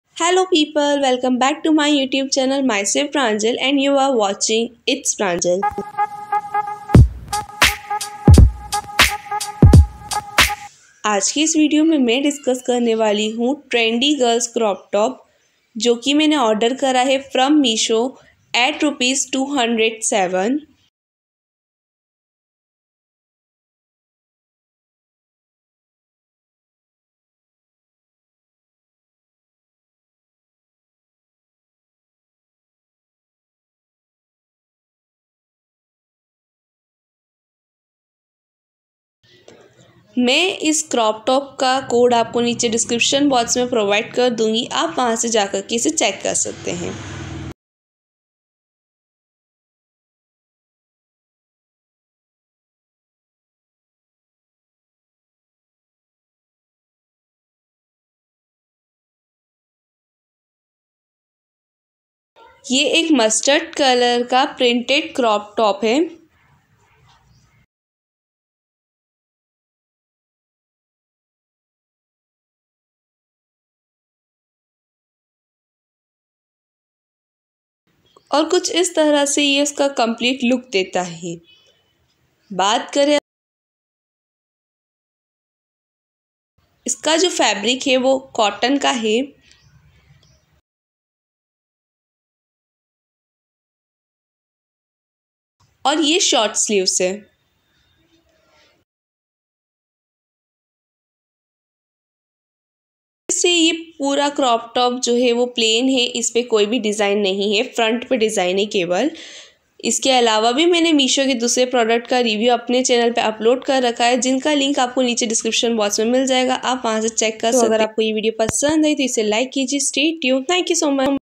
MEESHO || Trendy girls crop top review full video complete details💛
staytune for my upcoming videos💕
also do let me know about the things that you want me to review in the comment section💬😊👇

the code of meesho trendy girls crop top:-76950029

the link of meesho women lakhnavi chikankari cotton dress review complete detail video:-